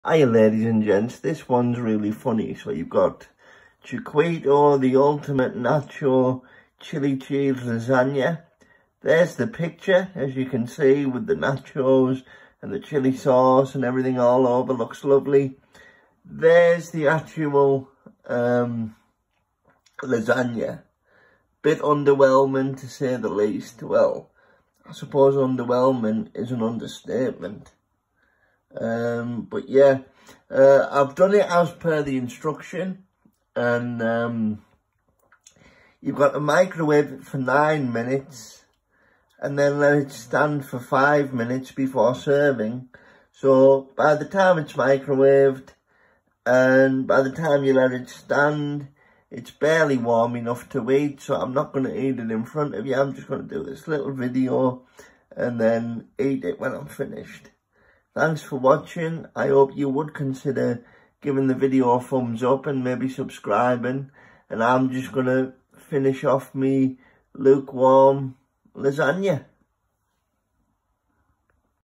Chiquito the ultimate Chili cheese lasagne ￼
I’ve resorted to trying ready meals in an effort to cut back on daily living expenses. I normally only eat fresh and I’ll have another video with the verdict of this meal￼.￼